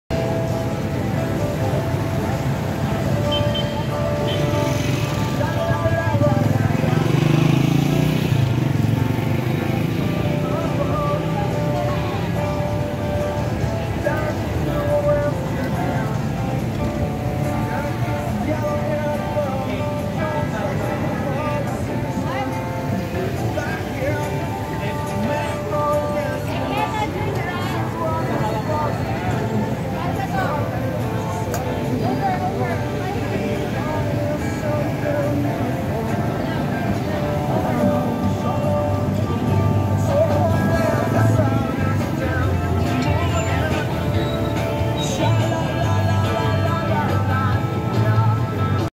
happy pamulinawen festival LAOAG CITY ILOCOS NORTE
Anga video na ito ay kuha noong feb28 2021 end of Ilocos norte pamulinawen festival.